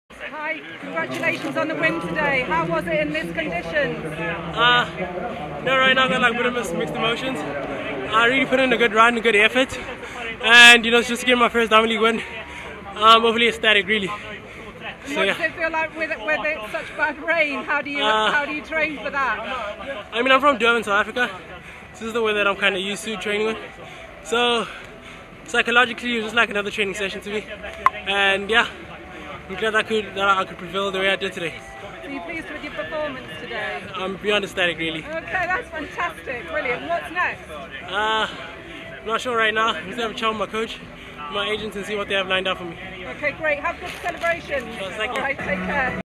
'Overly ecstatic!' - Zakithi Nene (RSA), Winner men's 400m (45.30) at Stockholm Diamond League 2023
"I'm overly ecstatic with the win," South Africa's Zakithi Nene after winning his first diamond league race, clocking 45.30 for victory in men's 400m in Stockholm on Sunday.

"I came into the race after a couple of days of sickness so that was very unexpected. To win my first Diamond League is very exciting in front of my friends and family.

"I am not sure what will be next as I need to sit down with my coach after today and analyse the race. I have been consistently running 45 lows which is great especially when the weather is bad like today.

"It was really terrible out there and quite tricky but I am from Durban so I am used it it; this is what I train in so maybe that helped me a bit."

(credit: Stockholm DL / Diamond league AG)